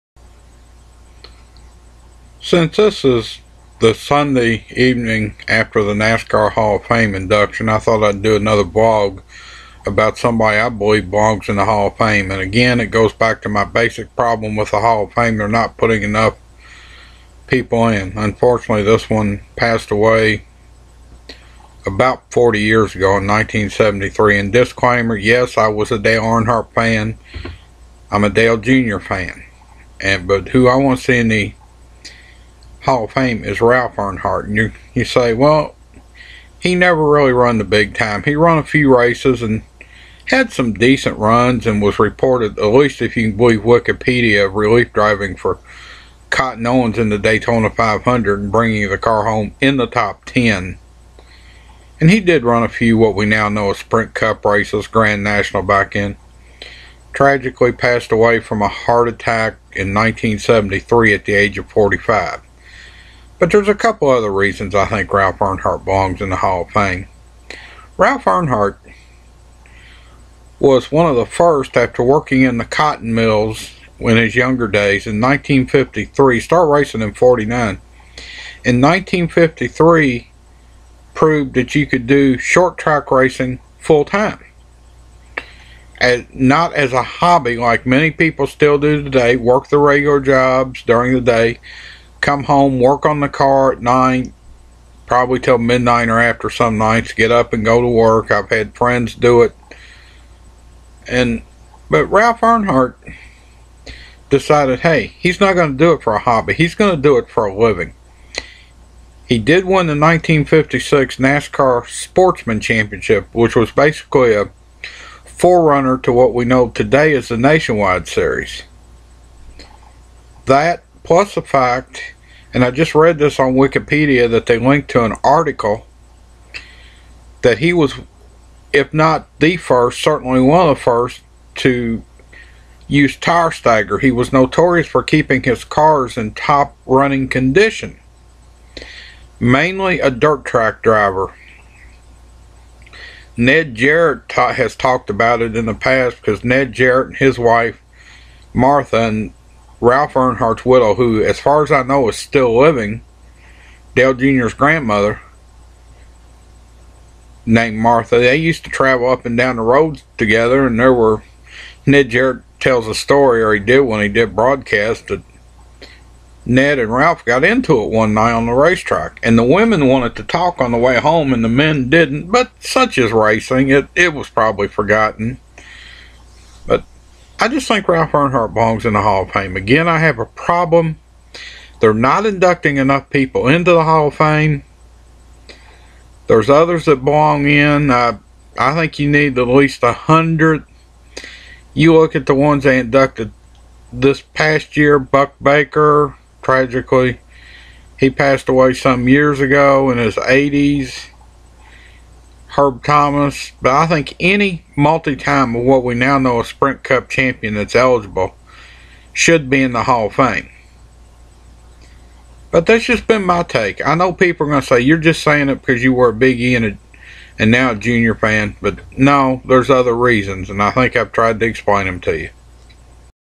Ralph Earnhardt for NASCAR HOF
In this vlog I tell you why Ralph Earnhardt belongs in the NASCAR HOF.